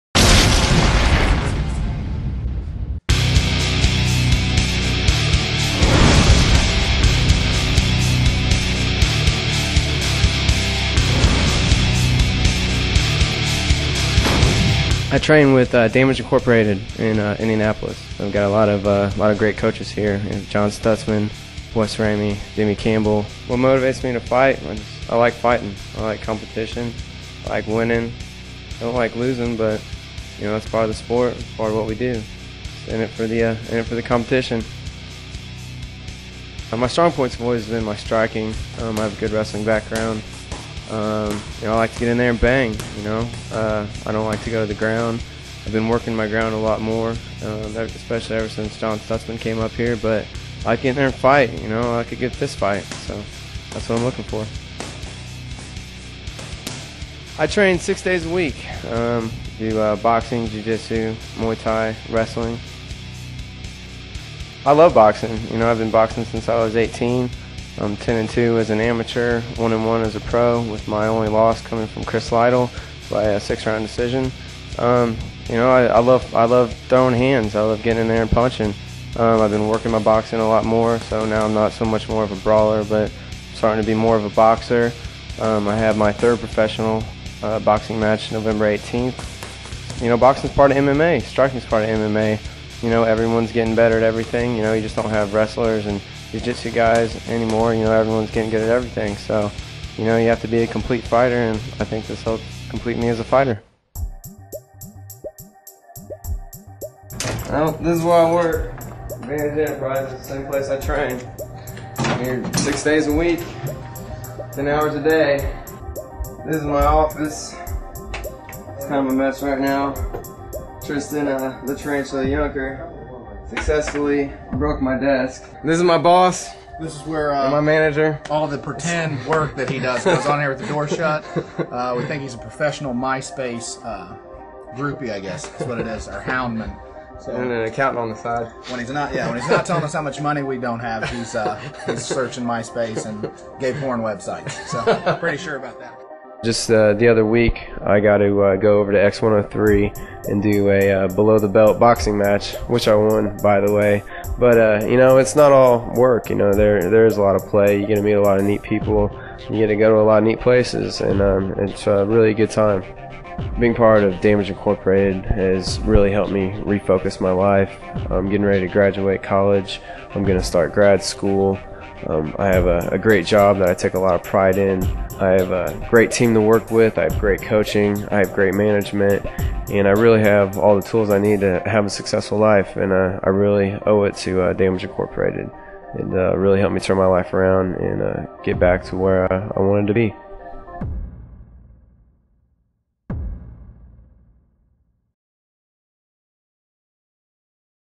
Tom Kirk Profile 2006
This is a profile of Tom "The Captain" Kirk. He is a fighter with Damage Incorporated in Indianapolis. Tom fights in the LFC and also in Boxing.